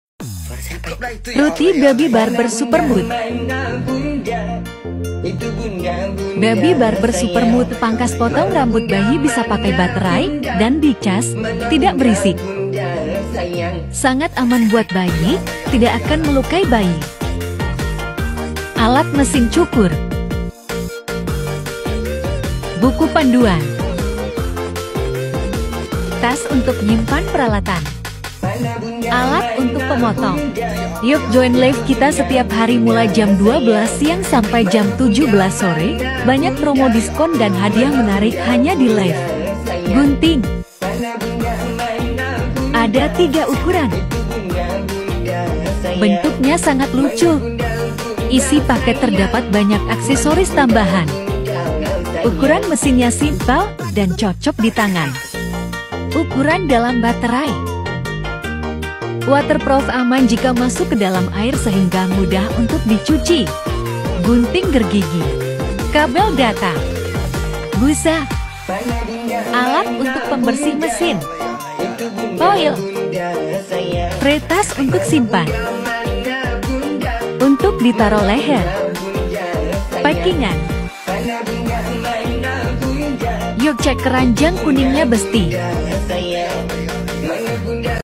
Ruthie Baby Barber Super Mute shaving hair rechargeable child shaving push Potong Pemotong Rambut
uthie Baby Barber Super Mute shaving hair rechargeable child shaving push Potong Pemotong Rambut

Baby Barber Super Mute shaving hair Clipper rechargeable child shaving push child baby home

Alat Mesin Cukur Pangkas Potong Rambut Bayi Baterai diCas, Tidak Berisik

Kelebihan Alat Pangkas Rambut Bayi ini
+ Mesin potongnya tidak berisik (sehingga anak/bayi tidak takut dan bisa dipakai saat bayi lagi tidur)
+ Sangat Aman buat Bayi , tidak akan melukai bayi
+ Waterproof aman jika masuk ke dalam air sehingga mudah untuk dicuci
+ Daya Listrik dari Charger / bisa di cas ulang (tidak perlu ganti baterai)
+ Packing Exclusive (cocok untuk jadikan hadiah)
+ isi paket terdapat banyak Aksesoris Tambahan
Free 2 gunting
Free Sisir Rambut
Free Tas Kecil untuk tempat simpan
(isi paket bisa lihat di foto)